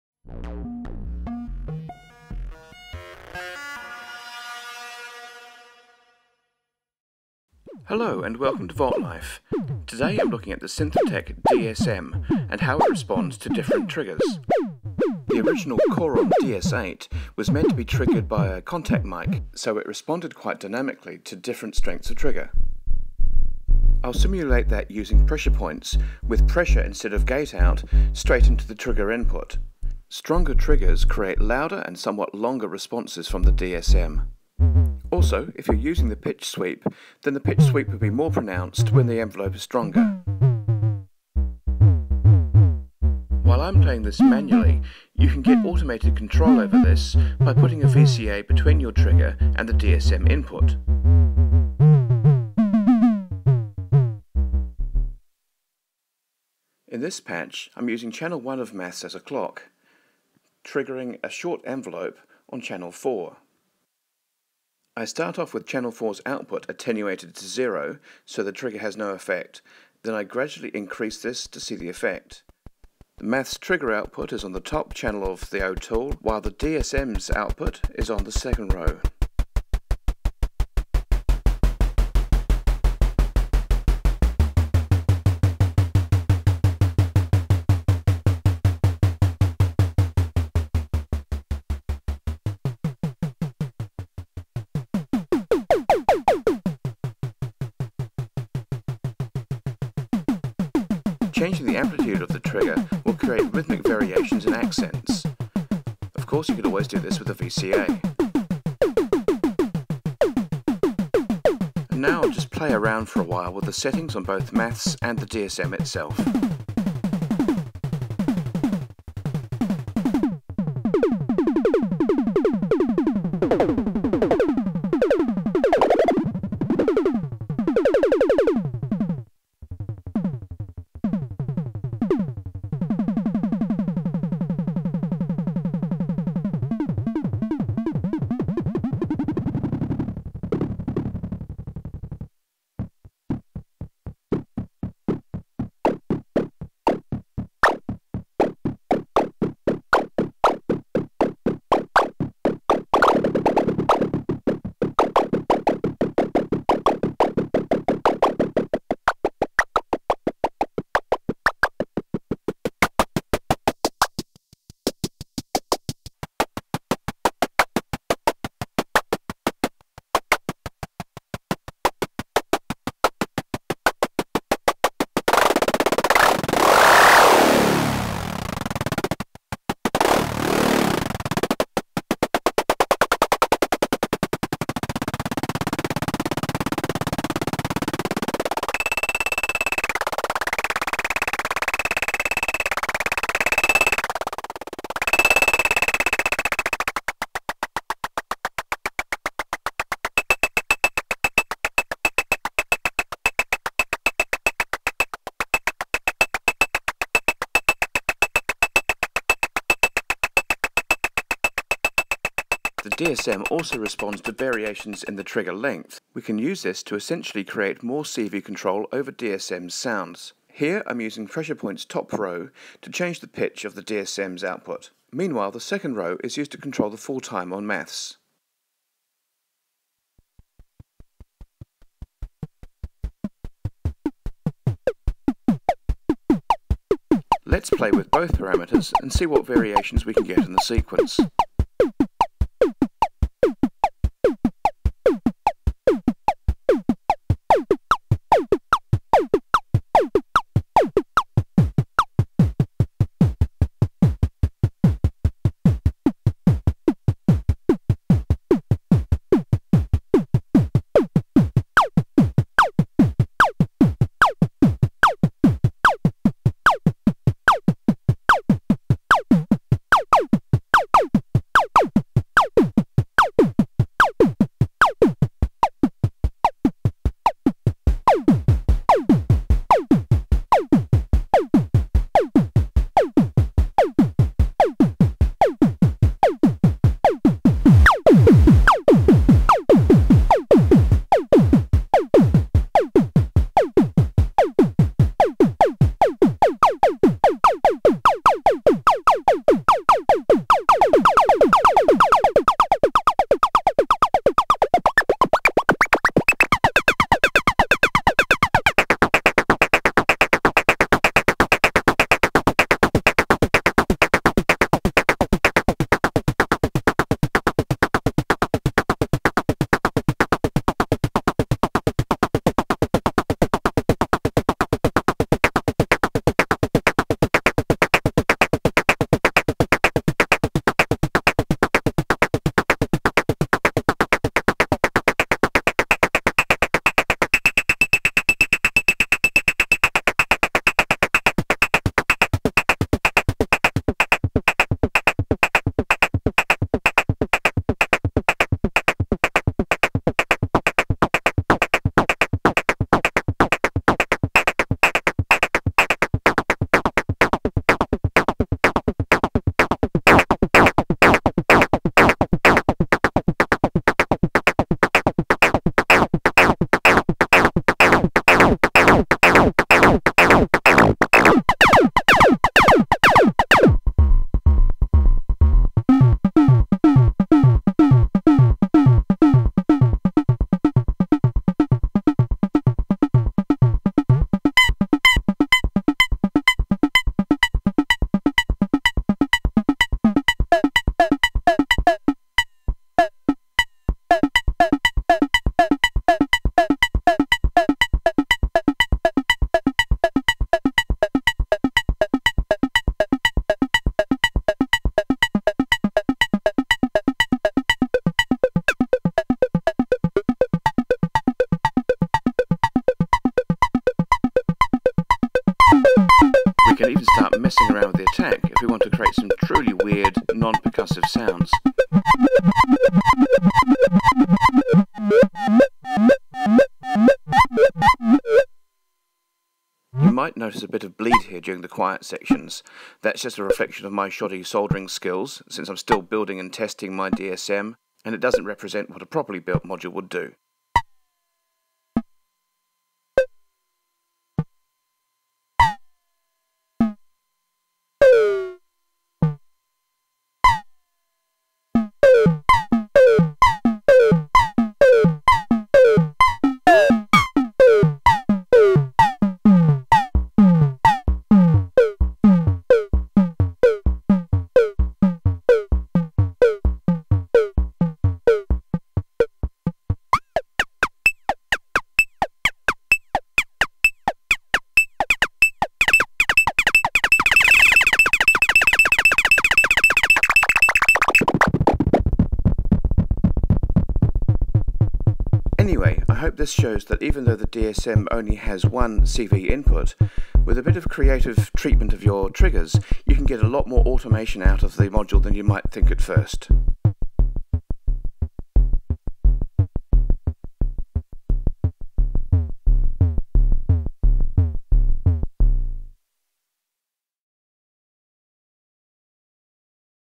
VOLTLIFE: DS-M variable trigger response
The Synthrotek DS-M drum module may only have one CV input, but it responds very dynamically to the amplitude and width of the input trigger. This tutorial shows how you can use this to get much more variety out of the DS-M's percussion sounds without having to manually tweak the knobs all the time.